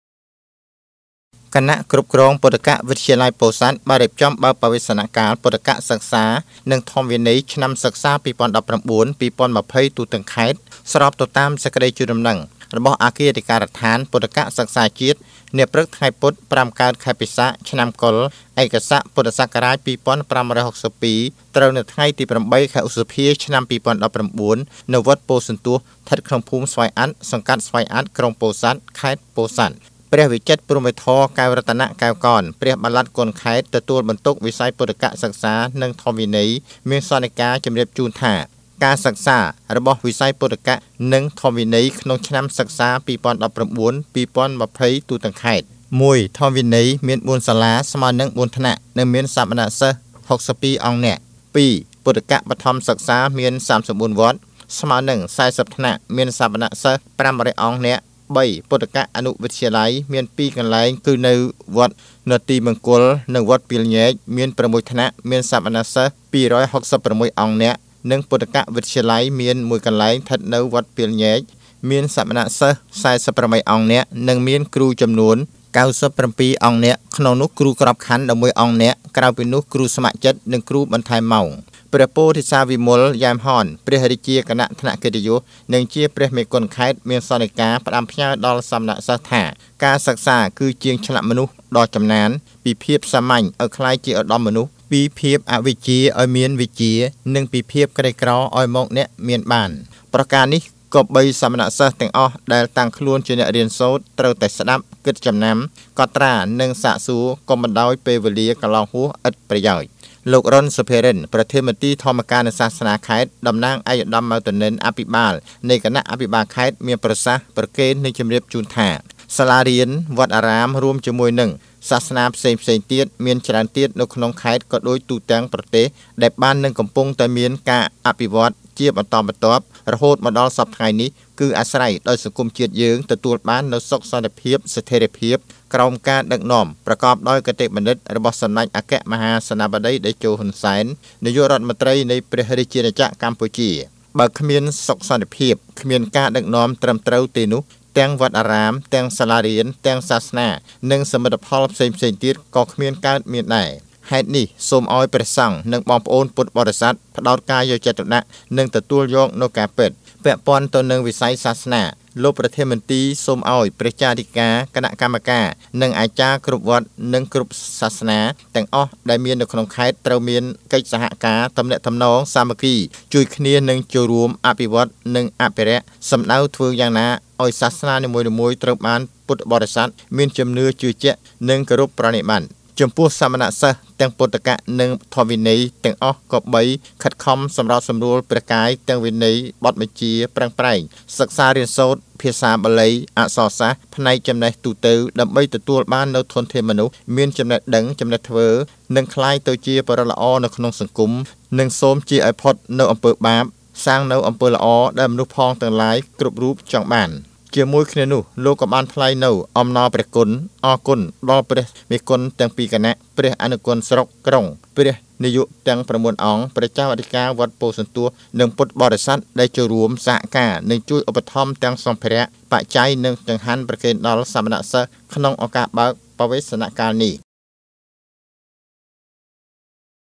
បើកបវេសនកាលពុទ្ធិកៈសិក្សានិងធម្មវិន័យឆ្នាំសិក្សា២០១៩-២០២០ទូទាំងខេត្តពោធិ៍សាត់
គណៈគ្រប់គ្រងពុទ្ធិកៈវិទ្យាល័យពោធិ៍សាត់បានរៀបចំបើកបវេសនកាលពុទ្ធិកសិក្សានិងធម្មវិន័យឆ្នាំសិក្សា២០១៩-២០២០ទូទាំងខេត្ត ស្របទៅតាមសេចក្តីជូនដំណឹងរបស់អគ្គាធិការដ្ឋានពុទ្ធិកសិក្សាជាតិ នាព្រឹកថ្ងៃពុធ ៥កើត ខែពិសាខ ឆ្នាំកុរ ឯកស័កព.ស២៥៦២ត្រូវនឹងថ្ងៃទី៨ ខែឧសភា ឆ្នាំ ២០១៩នៅវត្តពោធិ៍ទស្សន៍ ស្ថិតក្នុងភូមិស្វាយអាត់ សង្កាត់ស្វាយអាត់ ក្រុងពោធិ៍សាត់ ខេត្តពោធិ៍សាត់។